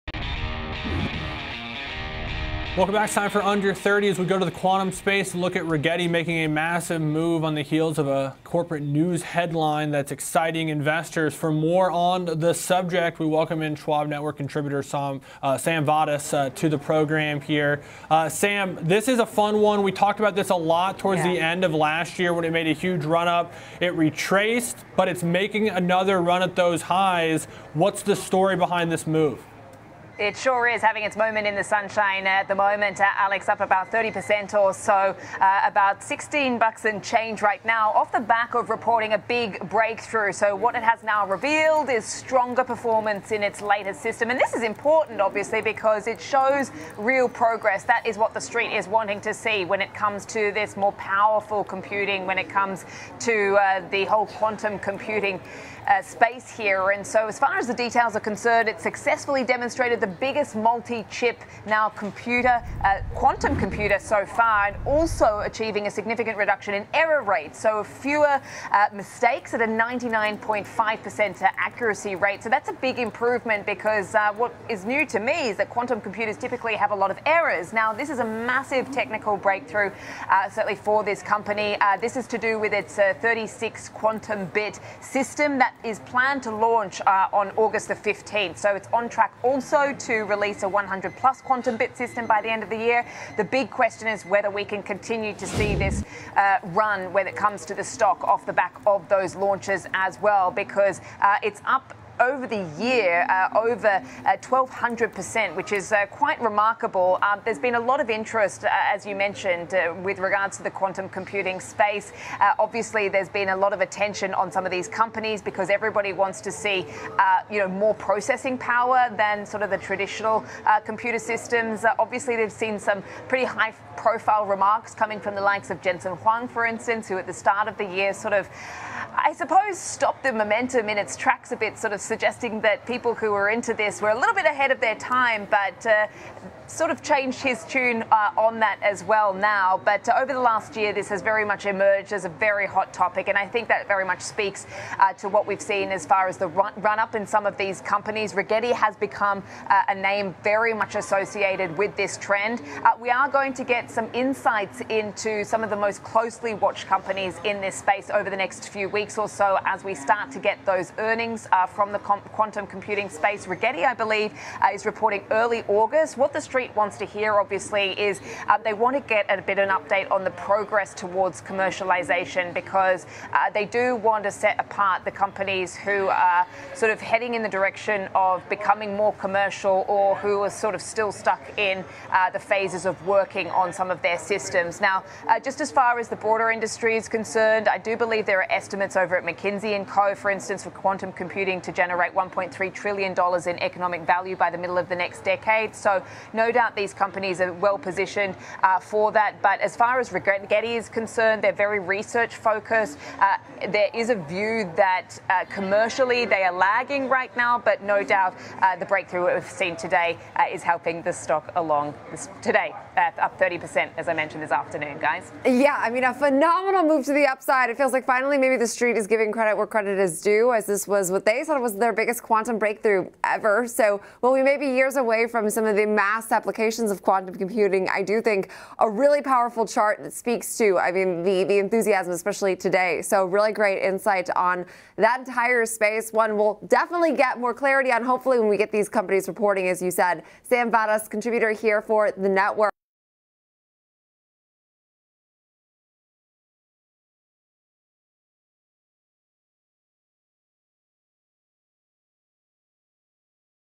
RGTI Rallies 30% Amid Quantum Computing Breakthrough
Rigetti Computing (RGTI) makes a quantum leap. The company announced that its 36-qubit quantum computer system reduced error rates in half. As the race for processing power intensifies, Sam Vadas talks about why Rigetti has become a name in the quantum space many are watching.